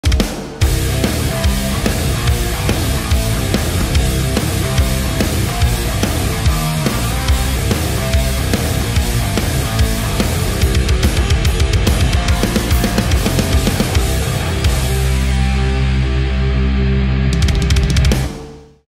PROMO Mediterranea Metal Cruise, 2022
Mediterranea Metal Cruise 2022 sailing on 4-7 November 2022
Lineup:
Stratovarius, Beast in Black, Ensiferum, Tankard, Enforcer, Myrath.